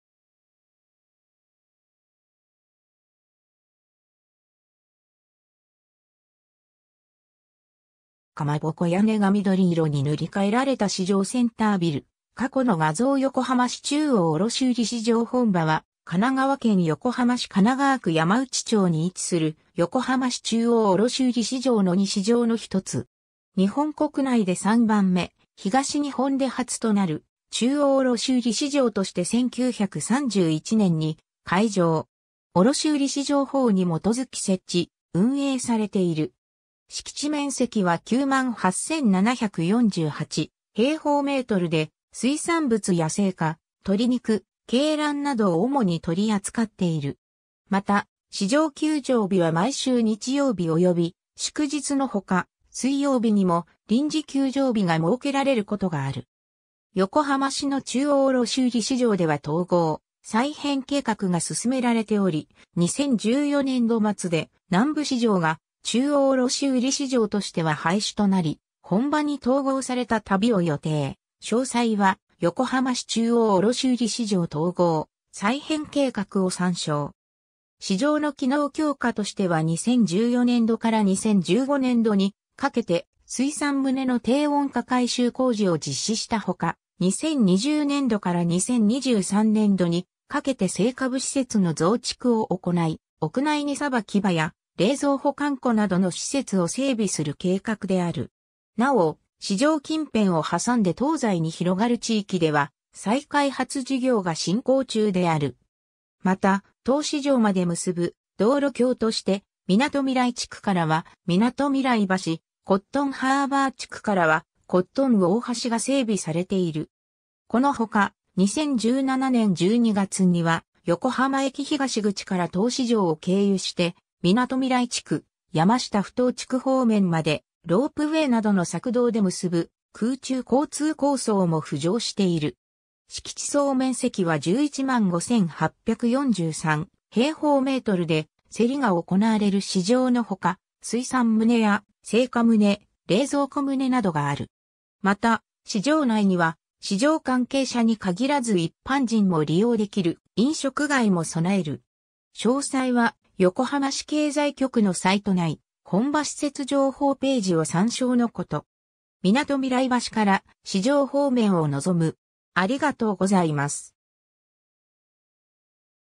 横浜市中央卸売市場本場
かまぼこ屋根が緑色に塗り替えられた市場センタービル（2017年5月14日）⇒過去の画像 横浜市中央卸売市場 本場（よこはましちゅうおうおろしうりしじょう ほんじょう）は、神奈川県横浜市神奈川区山内町（山内埠頭）に位置する横浜市中央卸売市場の二市場の一つ。
 日本国内で三番目、東日本で初となる中央卸売市場として1931年（昭和6年）に開場。
卸売市場法に基づき設置・運営されている。
敷地面積は98,748平方メートルで、水産物や青果、鳥肉、鶏卵などを主に取り扱っている。
また、市場休場日は毎週日曜日および祝日のほか、水曜日にも臨時休場日が設けられることがある。
 横浜市の中央卸売市場では統合・再編計画が進められており、2014年（平成26年）度末で南部市場（金沢区鳥浜町）が中央卸売市場としては廃止となり本場に統合された（統合に伴う全体の整備完了は2016年（平成28年）度を予定、詳細は「横浜市中央卸売市場#統合・再編計画」を参照）。
 市場の機能強化としては2014年（平成26年）度から2015年（平成27年）度にかけて水産棟の低温化改修工事（温度管理施設整備）を実施したほか、2020年（令和2年）度から2023年（令和5年）度にかけて青果部施設の増築（3棟）を行い、屋内荷さばき場や冷蔵保管庫などの施設を整備する計画である。
 なお、市場近辺を挟んで東西に広がる地域（コットンハーバー地区およびポートサイド地区）では再開発事業が進行中である。
また当市場まで結ぶ道路橋として、みなとみらい地区からは「みなとみらい橋」（歩道整備済み）、コットンハーバー地区からは「コットン大橋」（歩道未整備のため自動車等の通行のみ）が整備されている。
この他、2017年（平成29年）12月には横浜駅東口から当市場を経由して、みなとみらい地区、山下埠頭地区方面までロープウェイなどの索道で結ぶ空中交通構想も浮上している。
 敷地総面積は115,843m2で、せりが行われる市場のほか、水産棟や青果棟、冷蔵庫棟などがある。
また、市場内には市場関係者に限らず一般人も利用できる飲食街（食堂）も備える。
詳細は横浜市経済局のサイト内「本場施設情報ページ」を参照のこと。
 みなとみらい橋から市場方面を望む